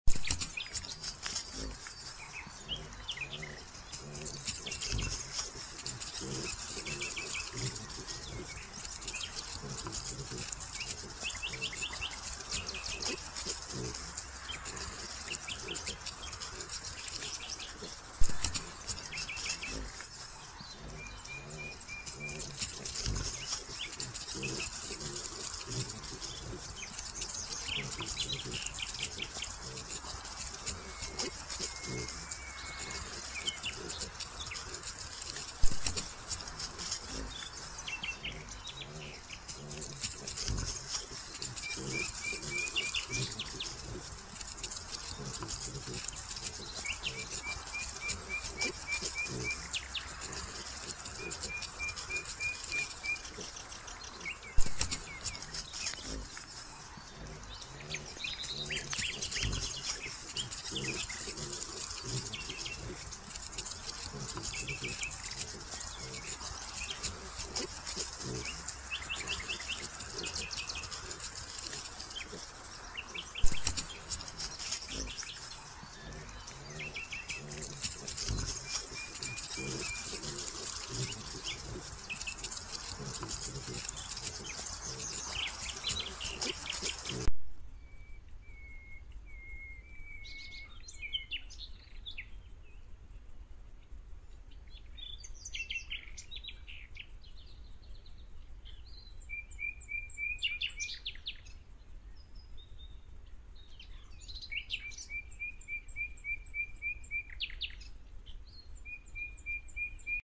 Videotrappolaggio - 4 lupi seguono i cinghiali (Ris.Nat.Marcigliana - Roma).
Lupi al crepuscolo mattutino seguono i cinghiali nella Riserva Naturale della Marcigliana - Roma.